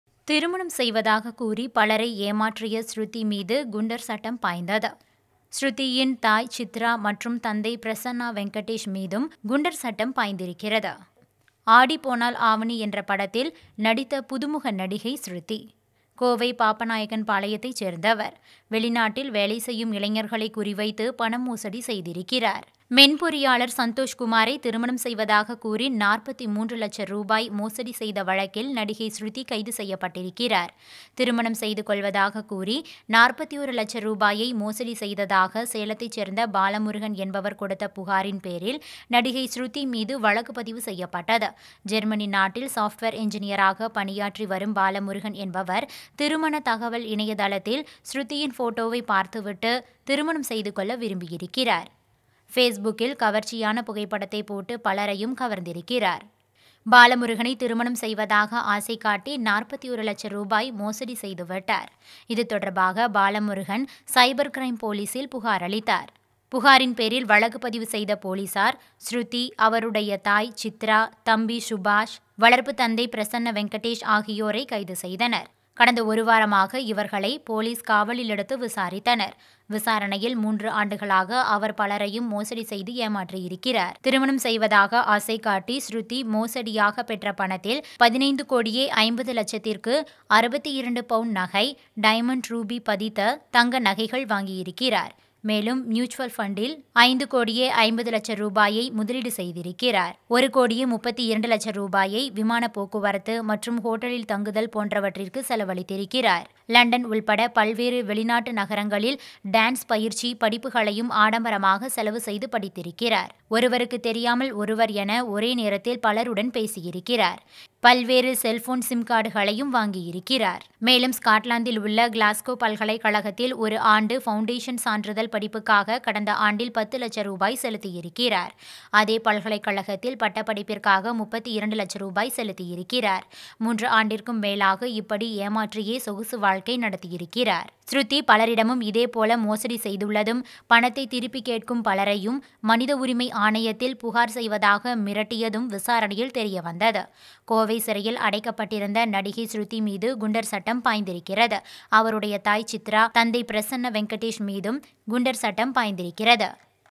திருமணம் செய்வதாக கூறி பலரை ஏமாற்றிய நடிகை ஸ்ருதி - Oneindia Tamil
திருமணம் செய்வதாக கூறி பலரை ஏமாற்றிய ஸ்ருதி மீது குண்டர் சட்டம் பாய்ந்தது. ஸ்ருதி தாய் சித்ரா மற்றும் தந்தை பிரசன்னா வெங்கடேஷ் மீதும் குண்டர் சட்டம் பாய்ந்துள்ளது.

ஆடி போனால் ஆவணி என்ற படத்தில் நடித்த புதுமுக நடிகையாக நடித்தவர் ஸ்ருதி. கோவை பாப்பநாயக்கன்பாளையத்தைச் சேர்ந்தவர். வெளிநாட்டில் வேலை செய்யும் இளைஞர்களை குறிவைத்து பண மோசடி செய்துள்ளனர்.

மென்பொறியாளர் சந்தோஷ்குமாரை திருமணம் செய்வதாக கூறி ரூ.43 லட்சம் மோசடி செய்த வழக்கில் நடிகை ஸ்ருதி கைது செய்யப்பட்டார்.

 The Coimbatore police have invoked provisions of Goondas Act to detain Conwoman Sruthi. She arrested recently along with her parents for cheating an NRI to the tune of Rs 41 lakh.The actress Sruthi allegedly concealed her original identity and allured the man for marriage.